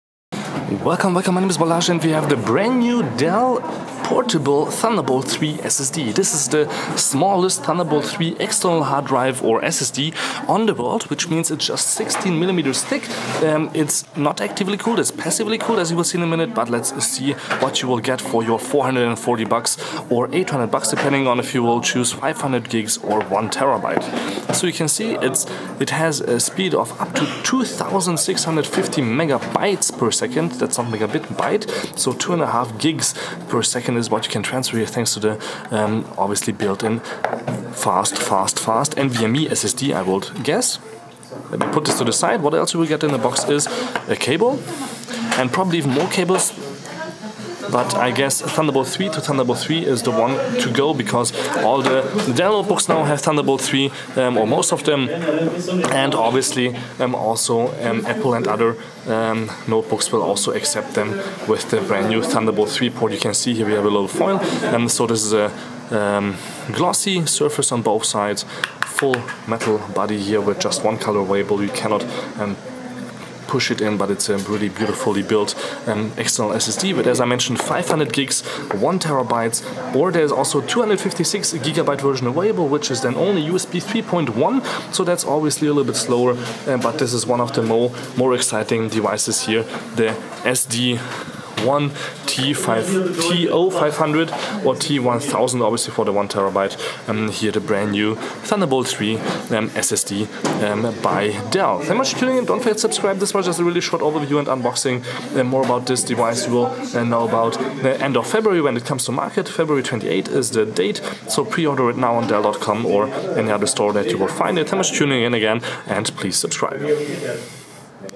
Smallest TB3 SSD in the World! | Dell Portable Thunderbolt 3 SSD Unboxing & Hands On
- February 28 the worlds smallest Thunderbolt 3 portable SSD will come to market, made by Dell. In a 16mm thick aluminium Case with no active Cooling you can soon transfer files as fast as 2650MB / Second. That is more than 5 times faster than USB 3.1 USB-C external Drives by SanDisk, Samsung, Plextor and other companies right now.

That is freakin´ fast but comes also with a hefty 449 US Dollar Price tag for 500GB and 799 US Dollars for 1000GB (1TB) of Storage space.
So better do not lose the brand new Dell Portable Thunderbolt 3 SSD Drive!

For everybody that is fine with 256GB of space and USB 3.1 in the same design, that will also come to market, but with an unknown price. The direct Amazon.com links will be added here as soon as possible:

This Video was shot in 4K 60FPS with the Panasonic Lumix GH5 and this 7-14mm lens